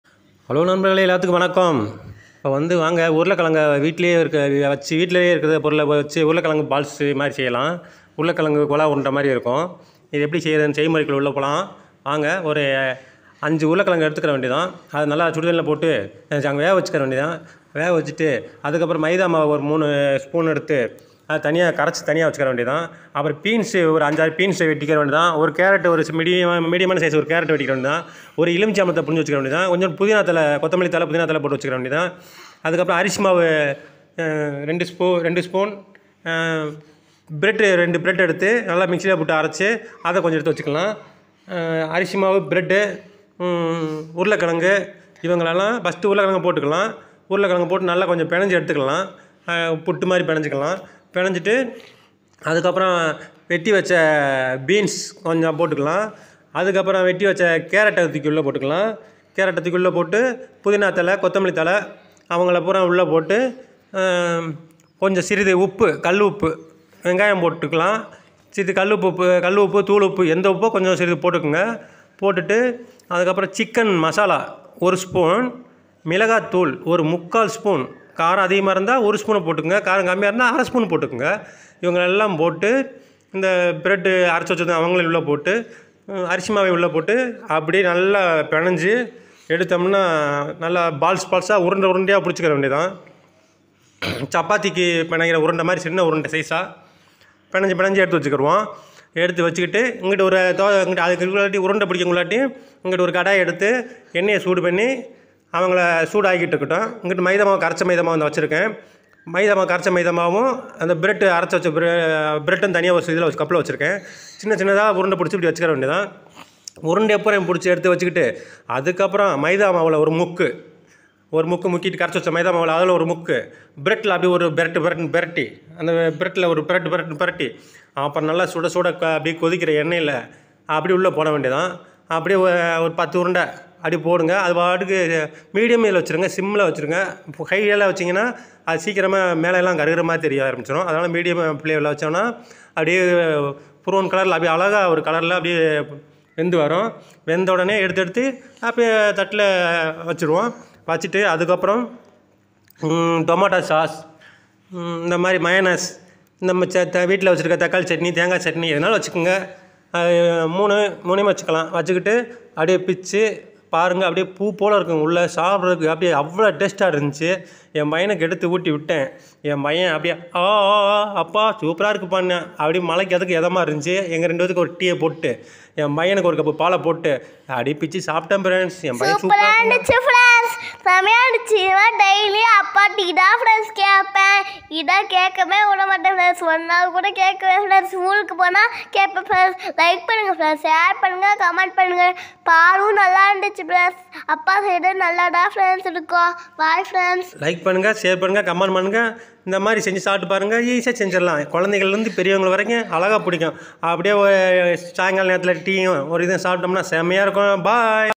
டீ போடுற நேரத்தில் இதை செய்து பாருங்கள் | tasty balls recipe with potato
Tasty balls with potato, bread crumbs, rice flour, maida, veggies, onion, chilli powder, salt, lime juice etc. The balls will be soft, yummy loved by all age peoples. It goes with tomato ketchup, chutneys.